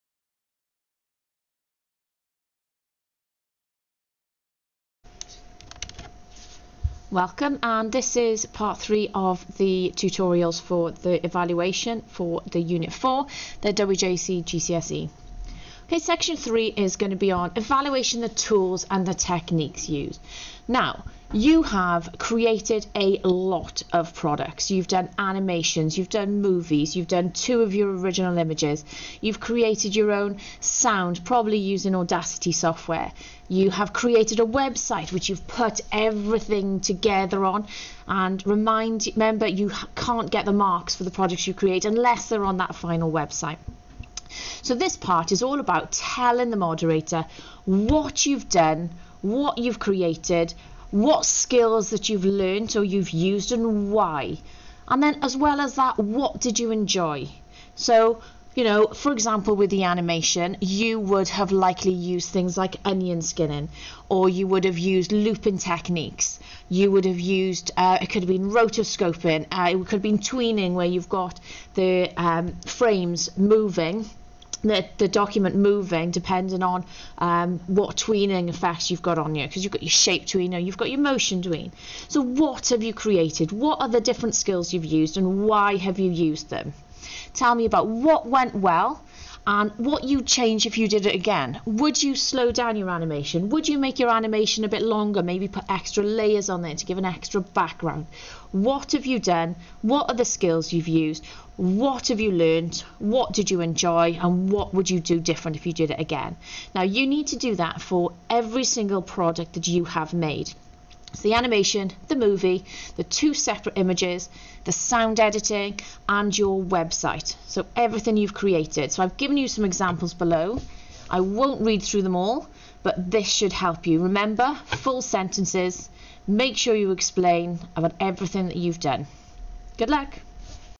GCSE ICT Unit 4 Evaluation Tools and Techniques 3
This tutorial is part of the GCSE ICT for Unit 4:

This document can be created at home on a PC, or can be drawn by hand.  If by hand, take a photo of it and submit it to your account  / to your teacher.